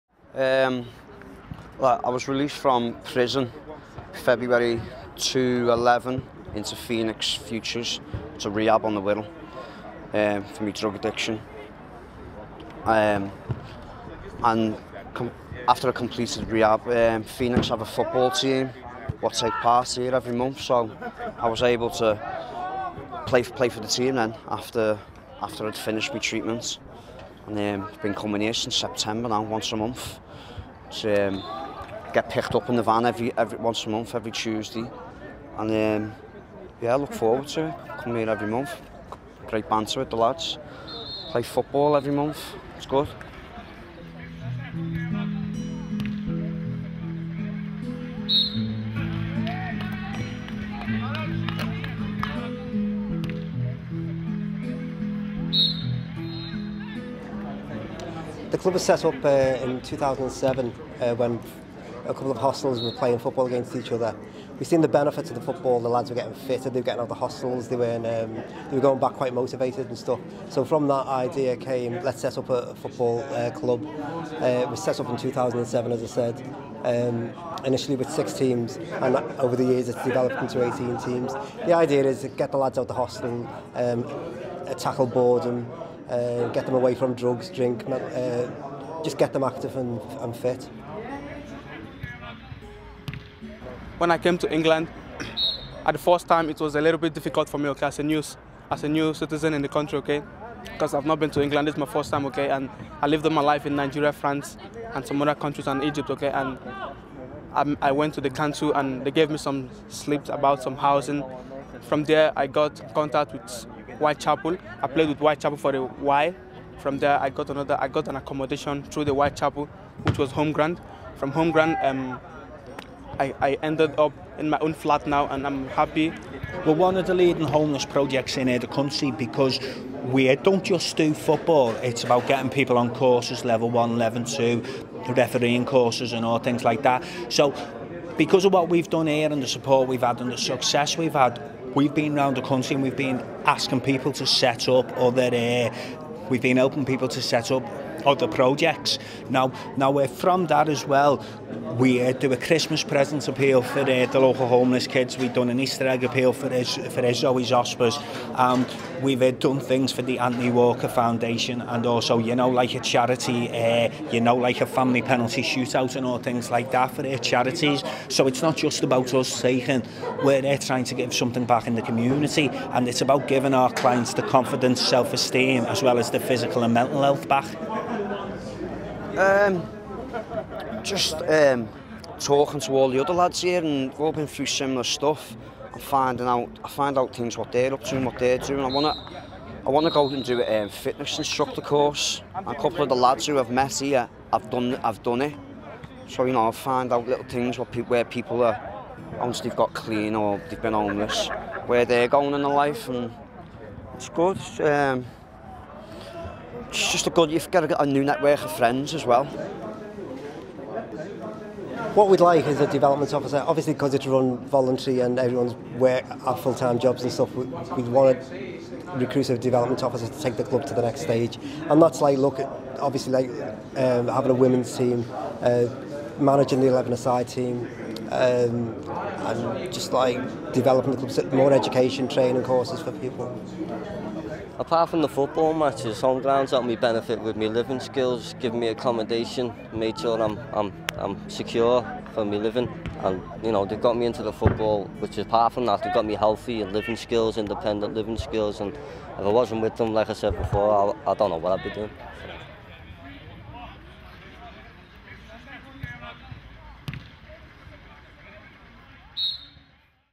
Liverpool Homeless Football Club
A video about local worthwhile project Liverpool Homeless Football Club, that tackles homelessness and promotes inclusion to help rebuild lives.